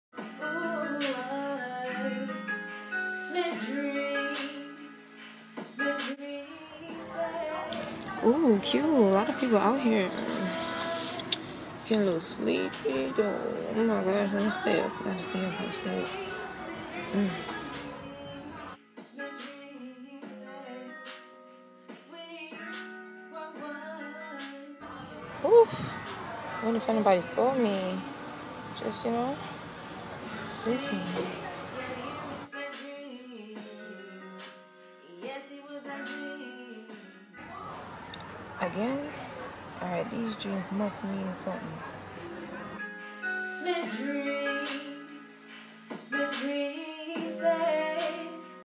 IPinkie learns French All Over Again...... Story 1: iPinkie Dreams To London •Episode 4
Final episode for this story! IPinkie learns French All Over Again.
Story 1: iPinkie Dreams To London
•Episode 4•
ITS iPINKIE 💕💋 Di JAMERICAN GYAL 🇯🇲🇺🇸•